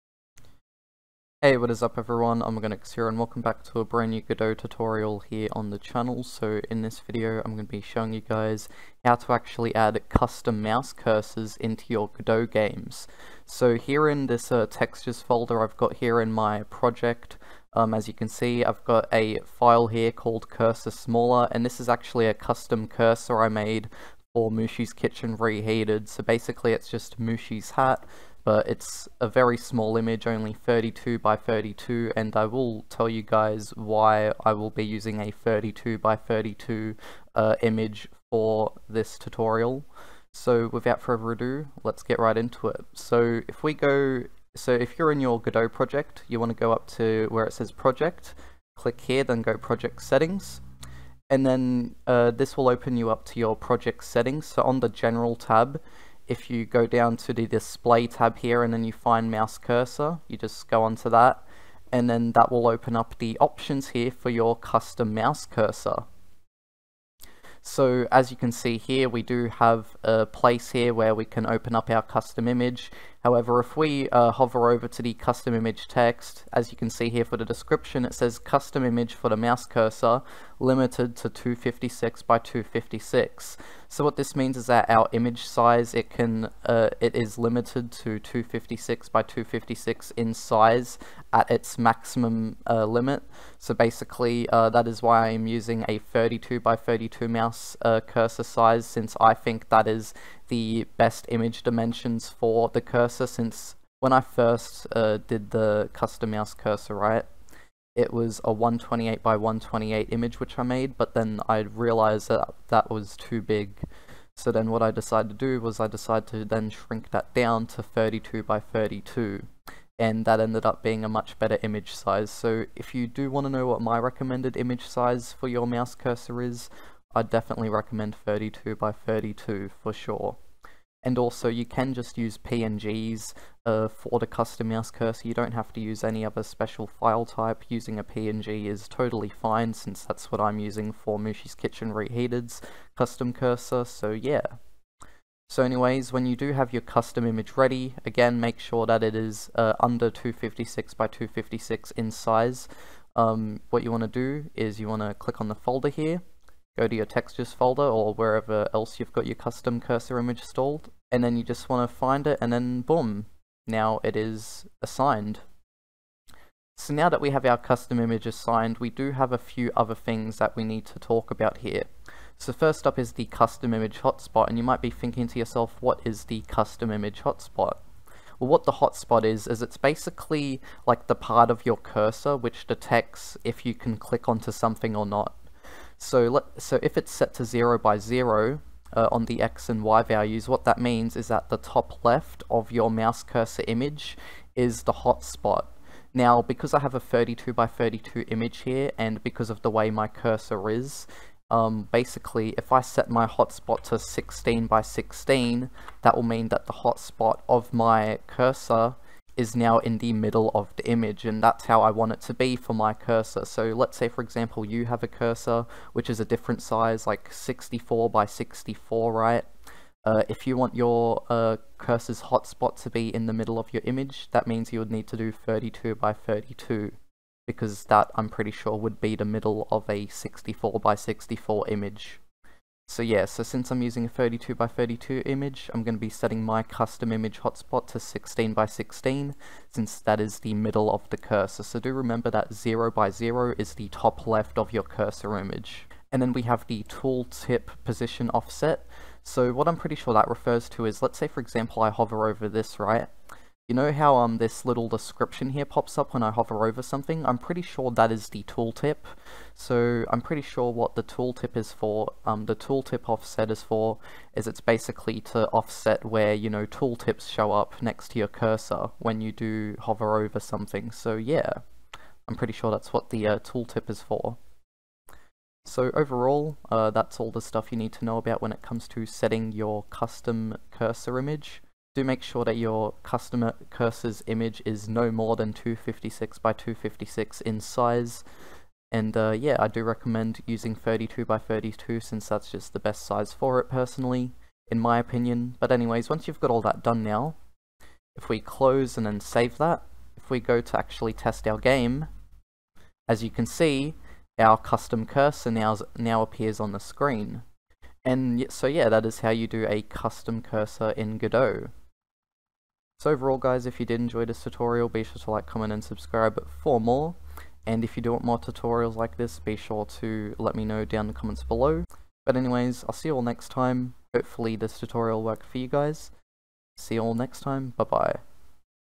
Custom Mouse Cursors in Godot - Godot 4 Tutorial
In this Godot 4 tutorial, I teach you guys how to add a custom mouse cursor to your game.